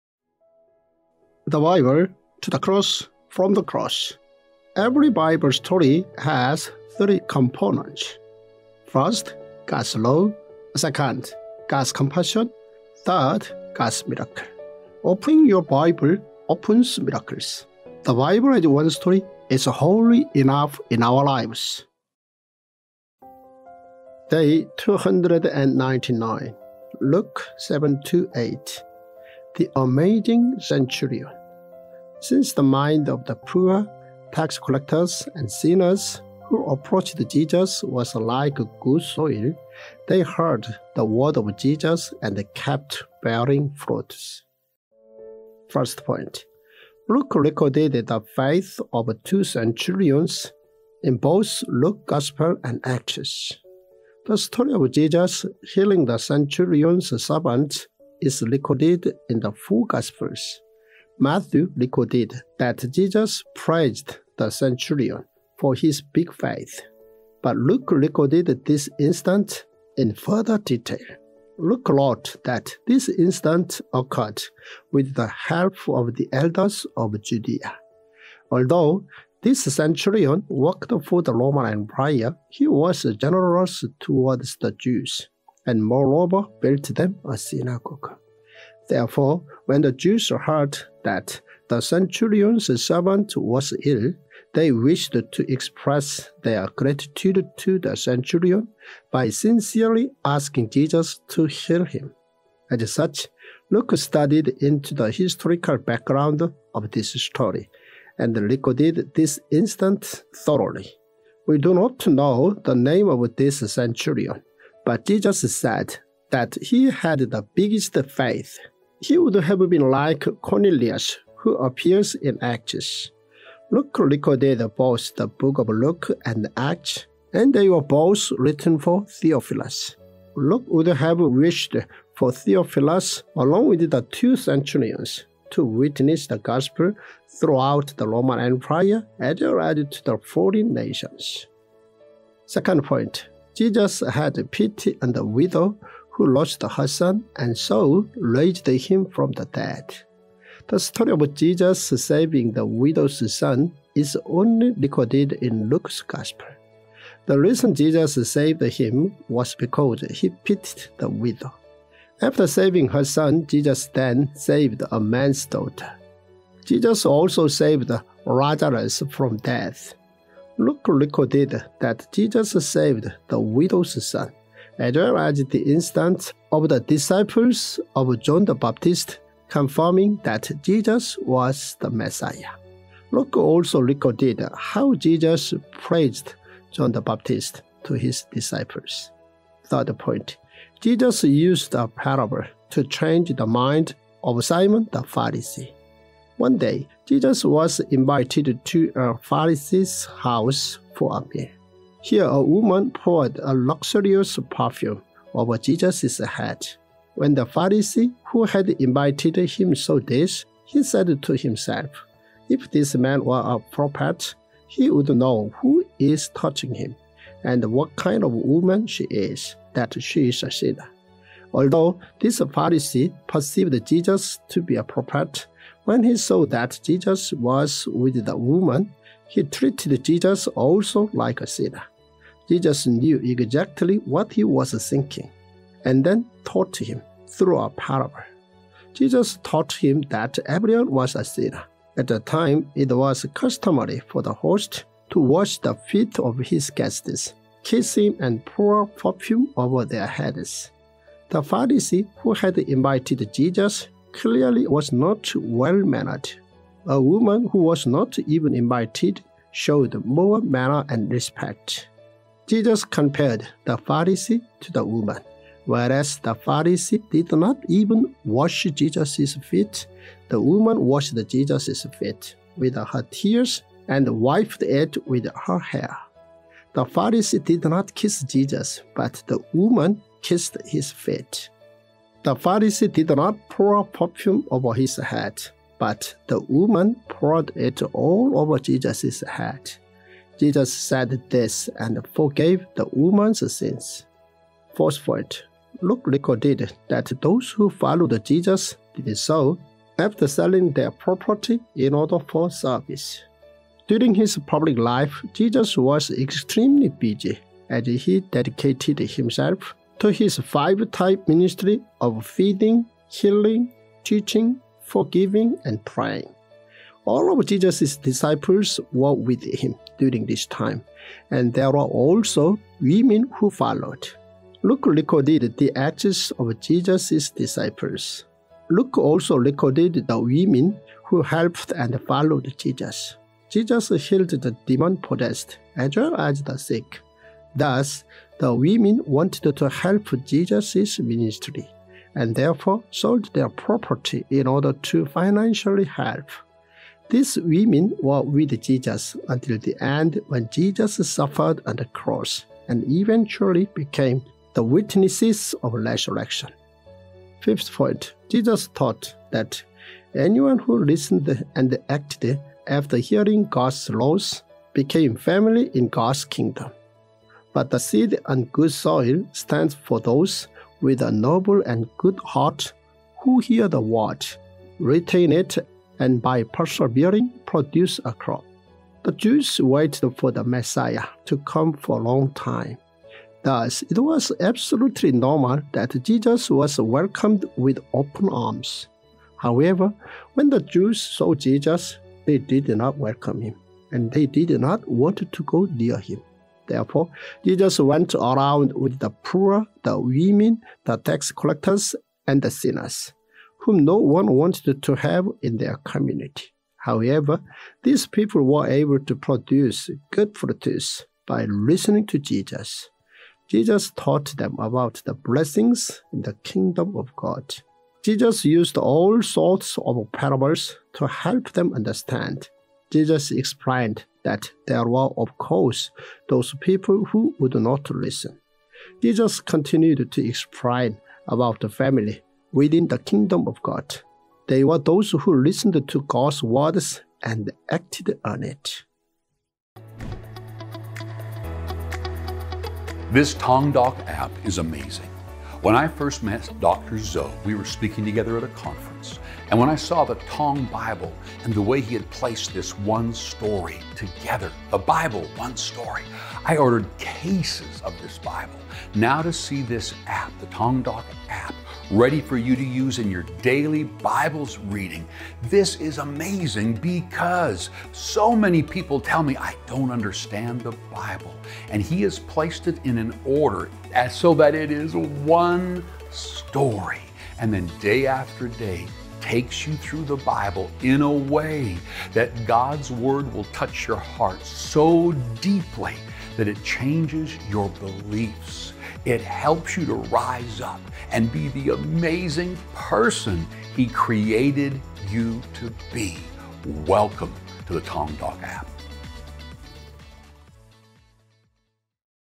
Day 299 Tongdok Bible | Luke 7-8 | The amazing Centurion | Dr.Byoungho Zoh
Tongdok Bible | Day 299 (Luke 7-8)

The amazing Centurion
Since the mind of the poor, tax collectors and sinners who approached Jesus was like good soil, they heard the word of Jesus and kept bearing fruits.